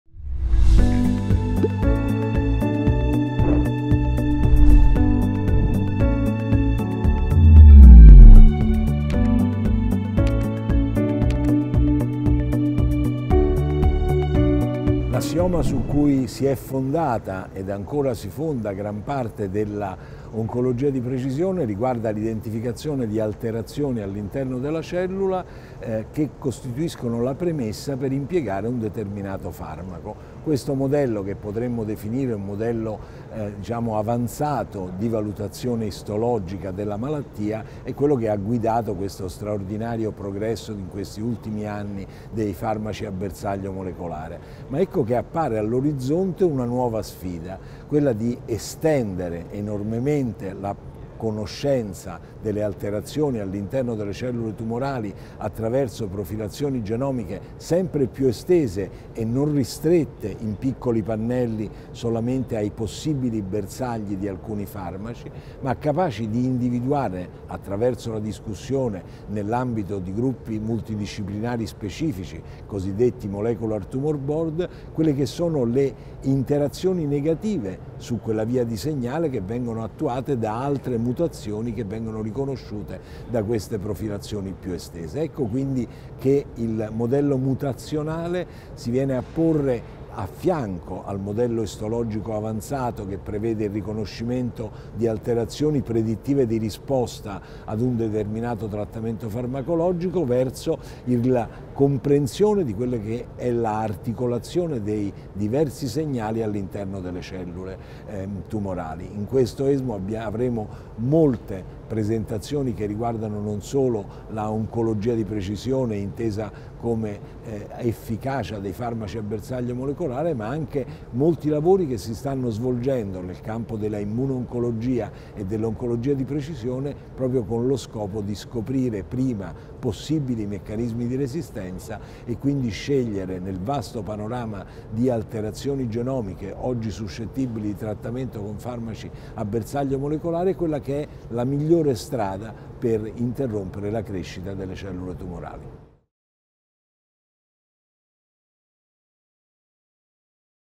Quale futuro per l'Oncologia di precisione?
[Oncoinfo – Istantanee di Oncologia: seguici su Ormai da qualche anno in ogni congresso si parla di Oncologia di precisione, ma da ESMO 2022 arrivano i primi segnali di un cambiamento che porterà questo approccio già innovativo di per sé verso nuovi orizzonti. Ne abbiamo discusso con Paolo Marchetti (Direttore scientifico IDI-IRCCS, Roma e Ordinario Oncologia, “Sapienza” – Università di Roma).